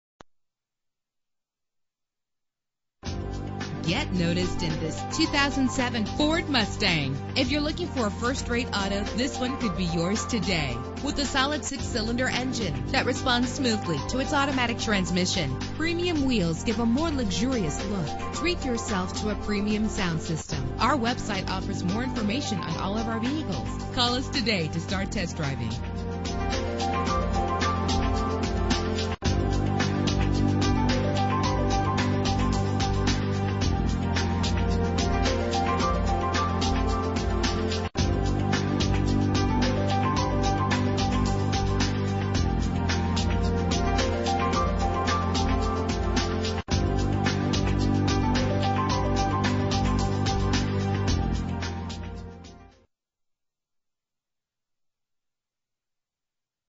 2007 Ford Mustang - Glendale AZ
Year: 2007
Make: Ford
Model: Mustang
Trim: Deluxe
Engine: 4.0 liter 6 cylinder 12 valve
Transmission: Automatic
Color: Red
Mileage: 45658
Address:
6400 North 51st Avenue